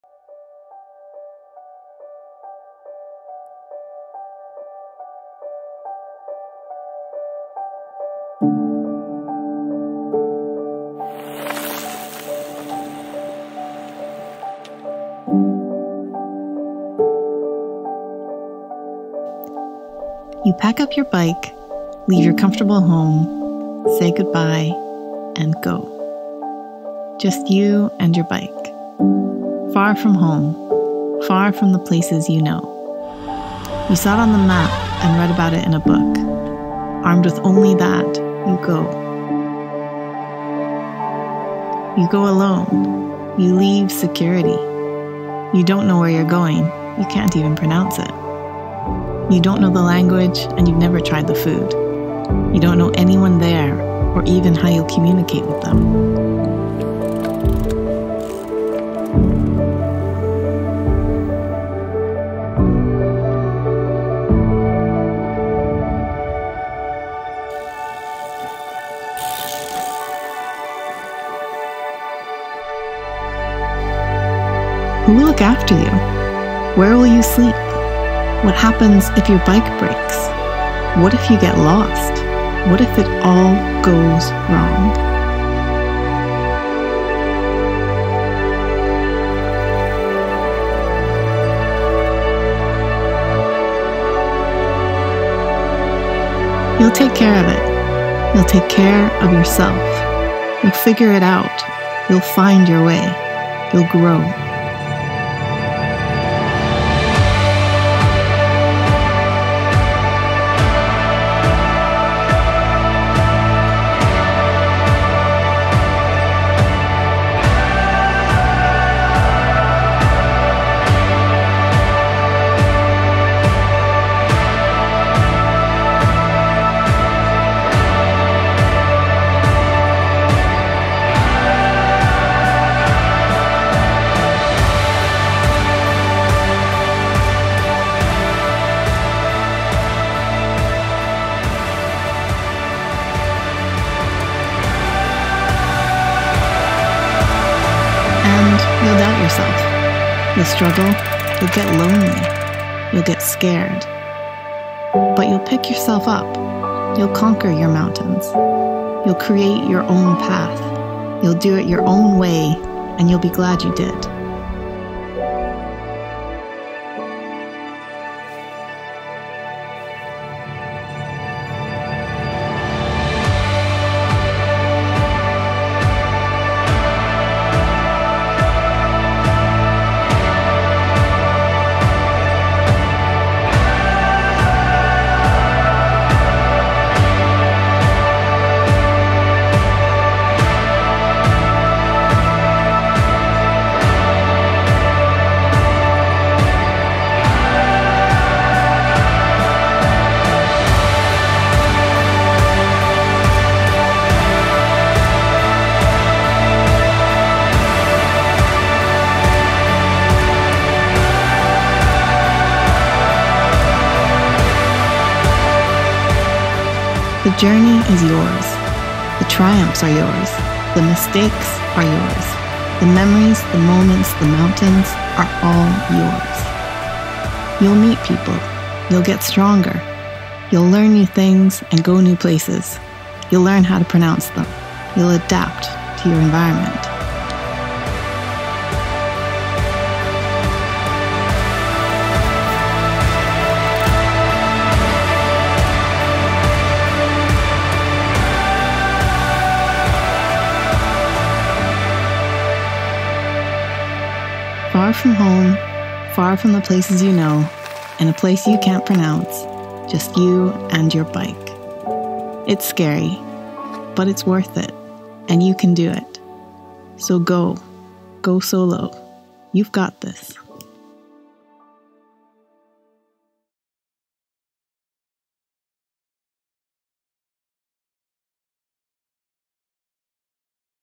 Go Solo - You've Got This (Bikepacking Nepal by Jenny Tough)
Travelling solo is an incredibly rewarding experience, but it can be scary to start. I did my first solo bike adventure when I was 21, and while I had absolutely no idea what I was doing and broke down (physically, mechanically, emotionally, etc.) a lot, it was one of the most important growth experiences of my adult life.

This December I went to the Nepalese Himalayas on my Shand Bahookie for a solo adventure. It didn't all go to plan, but I had an amazing time. And it was still an important growth experience. It always is.